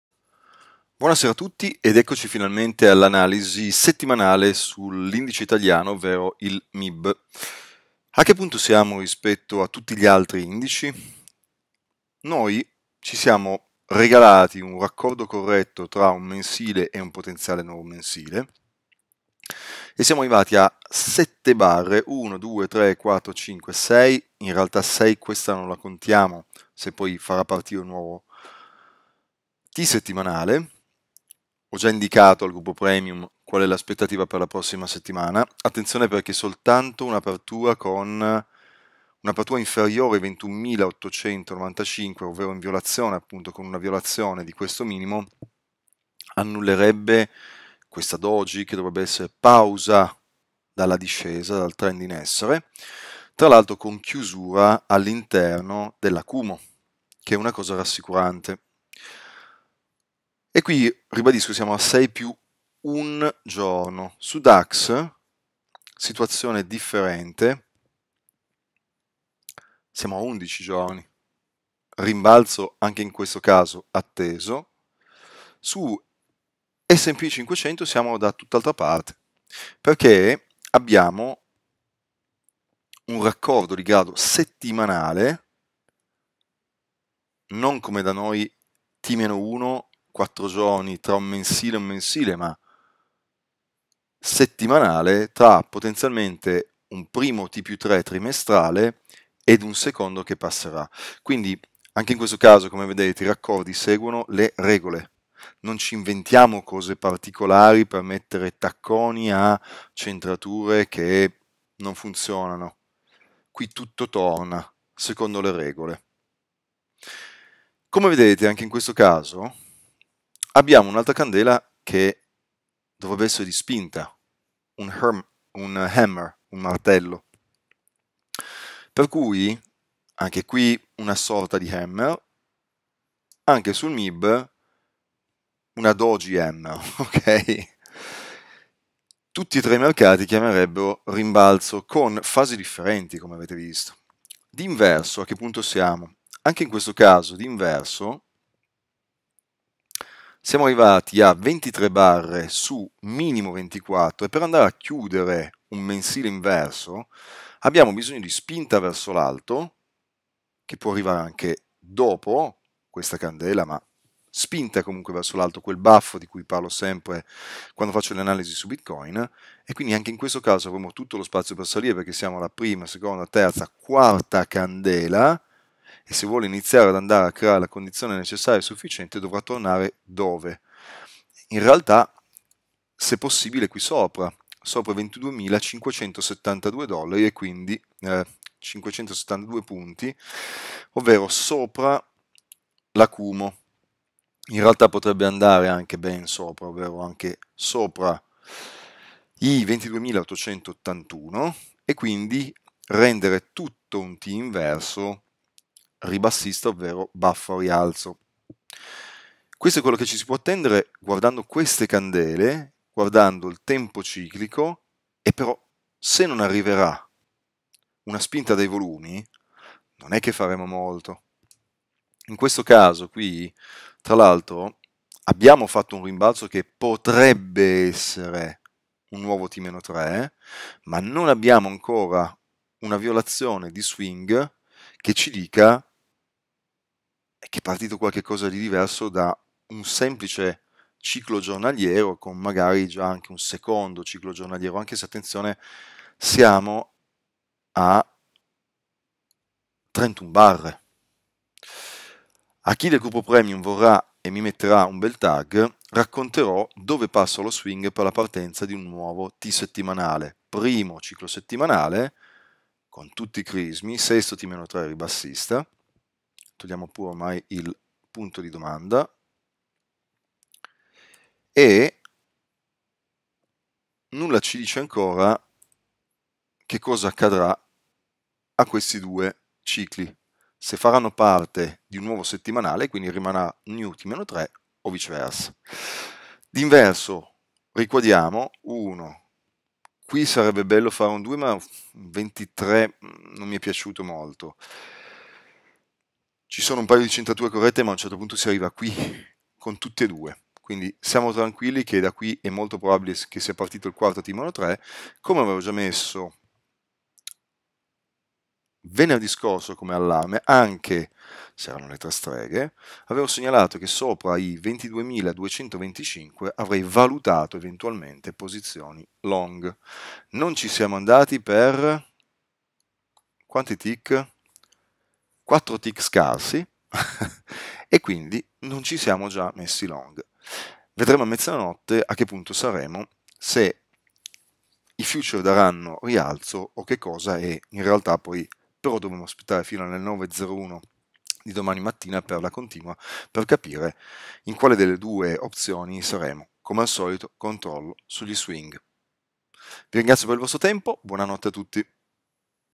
MIB - Analisi Evoluta Weekly del 18/9 - Attesa per un rimbalzo
Buona sera,

Perchè ci aspettiamo una ripartenza a rialzo sul MIB?
Per tanti motivi illustrati nella video analisi.
Solo con gli swing ci muoveremo per il trading.

Buon ascolto e ri ascolto.

Jacopo Marini